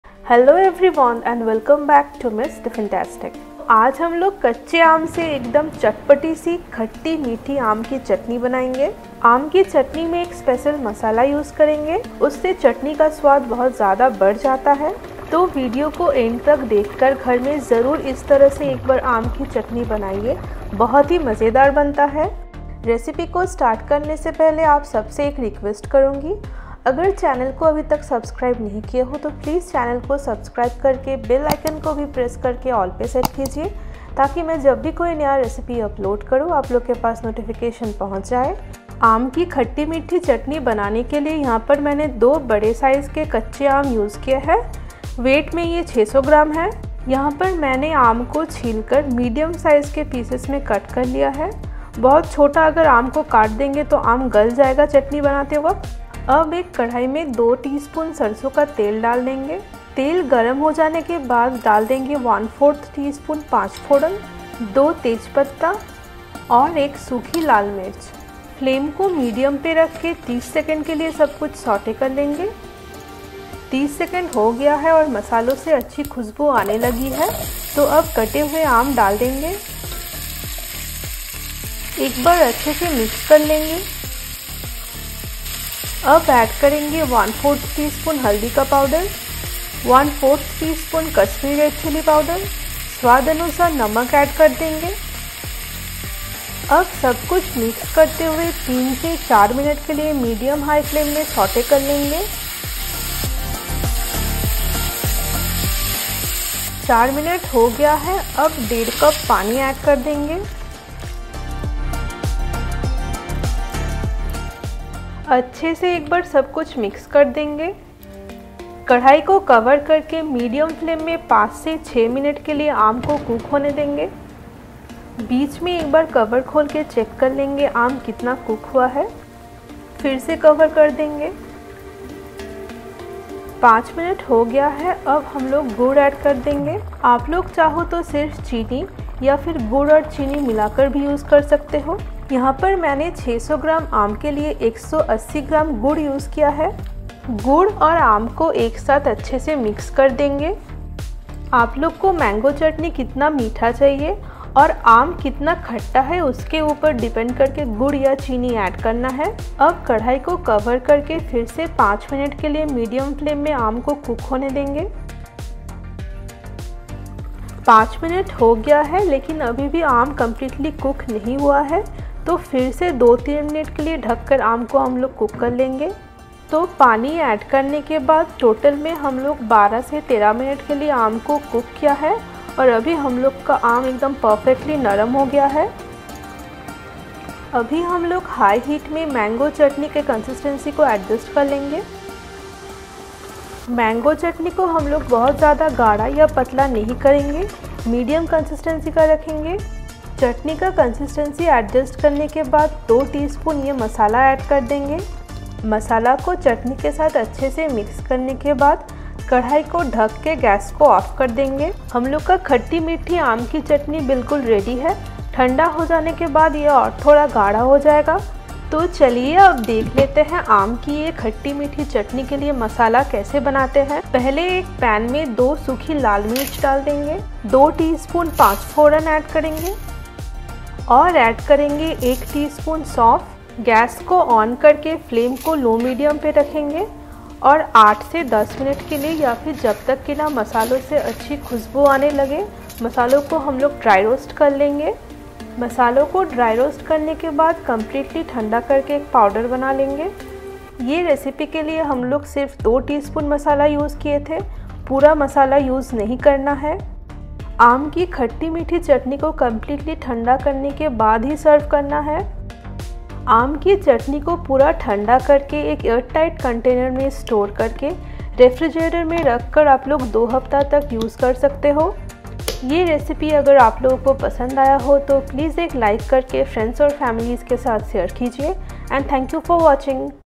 How to make Mango Chutney | Kacche Aam ki Chatpati Khatti Meethi Chatni
Hello Friends! Welcome to mstiffintastic
Today I have shared a mango chutney recipe / aam ki khatti meethi chutney

Ingredients  :
Mango - 600gm
Jaggery / Gur - 180gm or as required
Panch Phoron / blend of 5 spices - 1/4 tsp
Water - 1 1/2 cup or as required
Prepared dry roasted masala - 2 tsp

In this channel I will be sharing Tested and Tried recipes. Recipes that I prepare for my Family